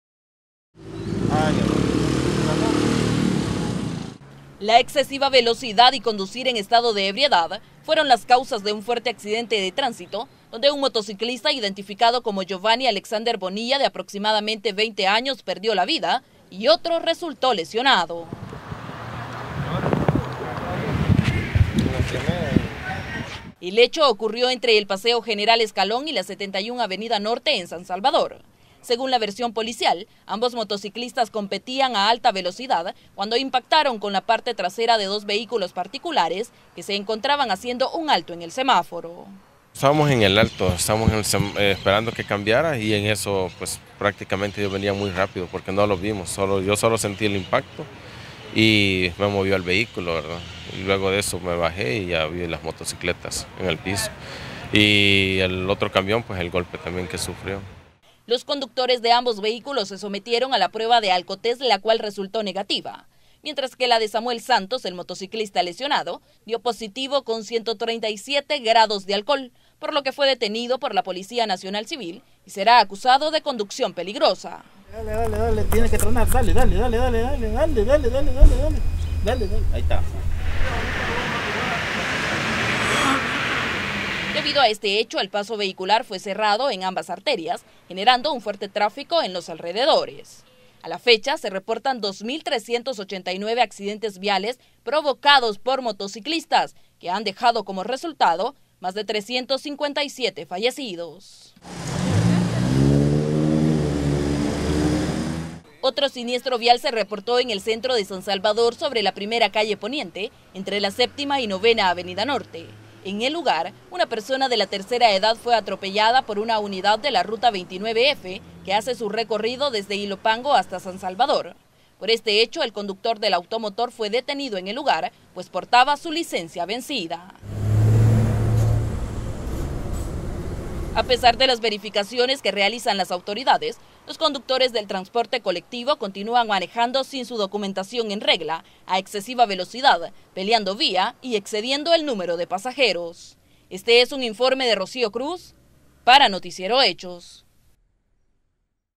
Un motociclista fallecido en accidente de transito
Un motociclista fallecido y otro lesionado fue el resultado de un accidente de tránsito registrado en el Paseo General Escalón.